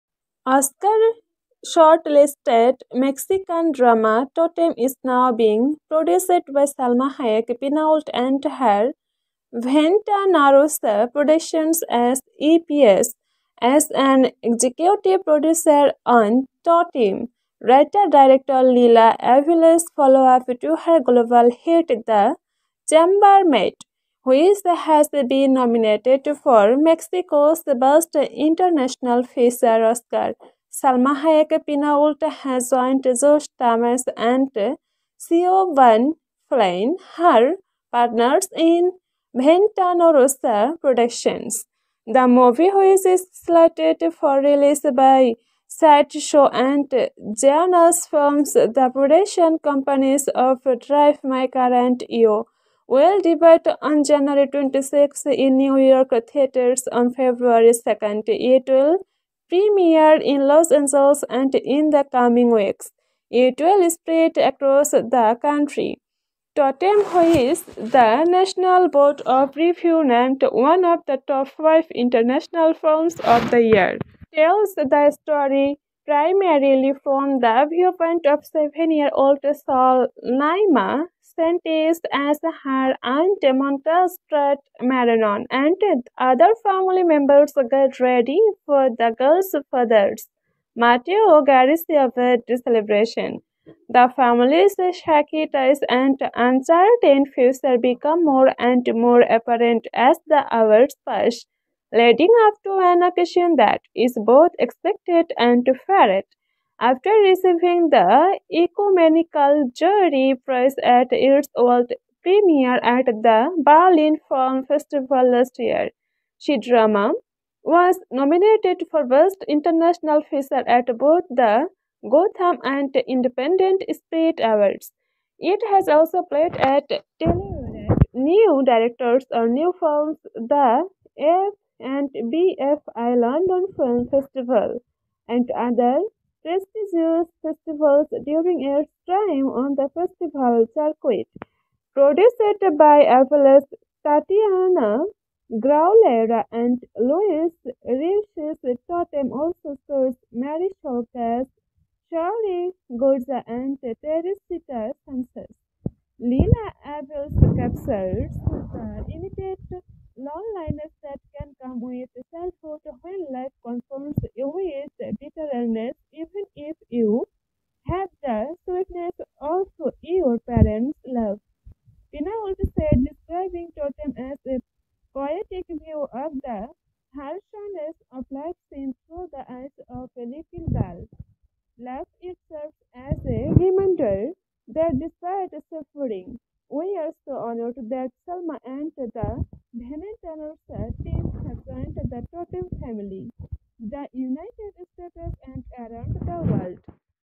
Salma Hayek's New Venture: EP for Oscar-Shortlisted Mexican Drama 'Tótem'! 🌟🎬
Good bye